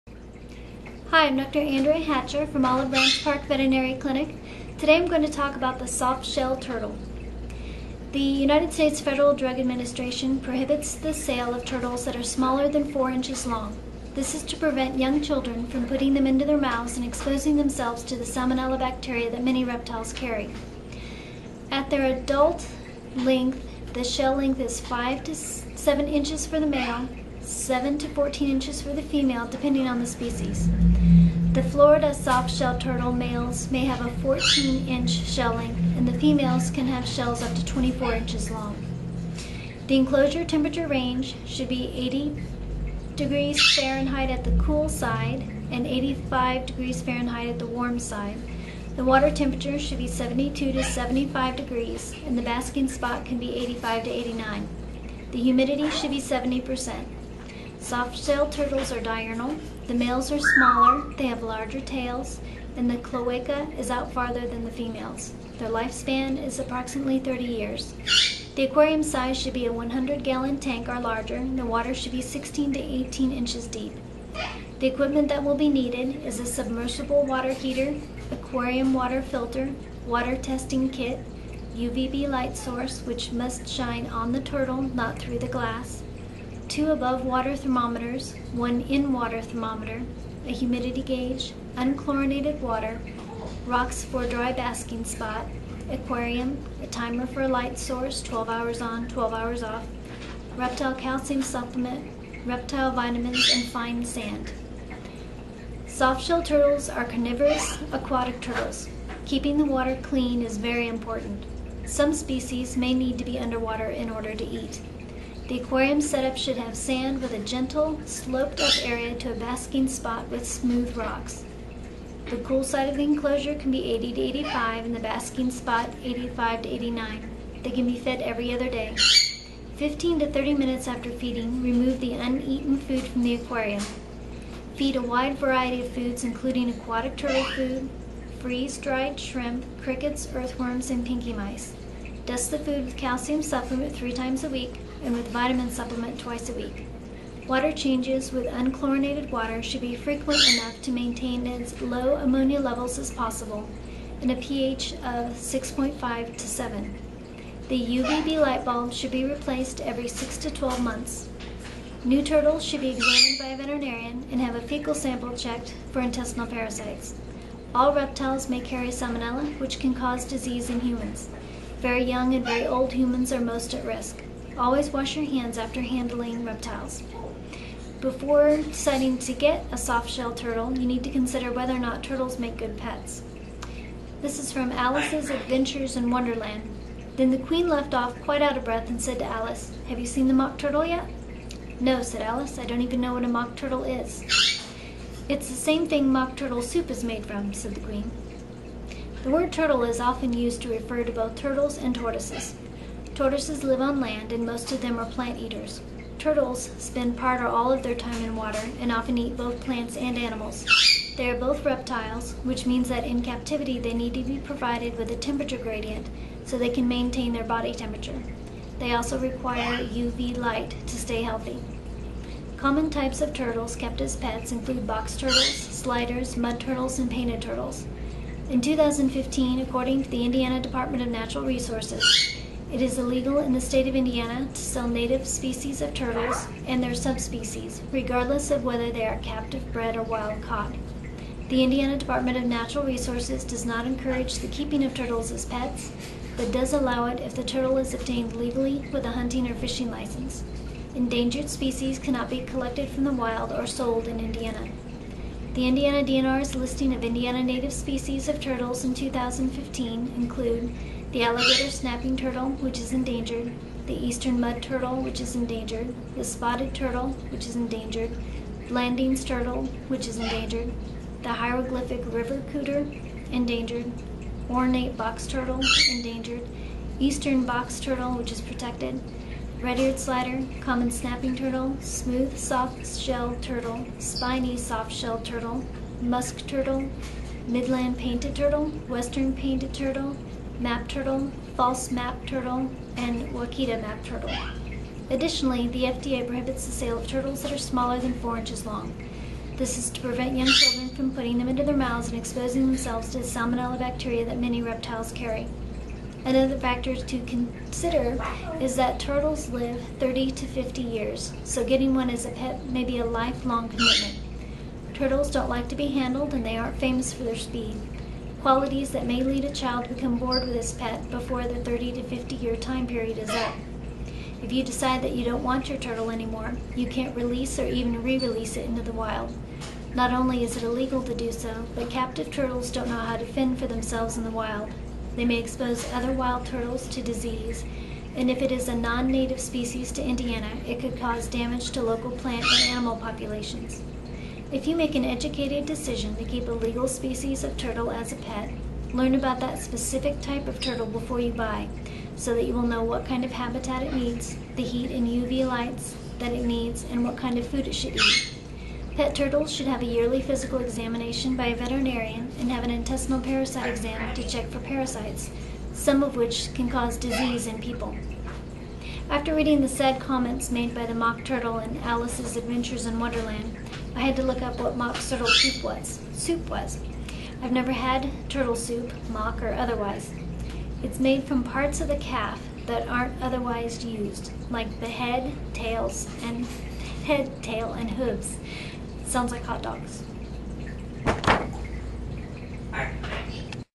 Soft Shelled Turtle
This is an informational video about the Soft-Shelled Turtle.

Dr. Anndrea Hatcher

Olive Branch Parke Veterinary Clinic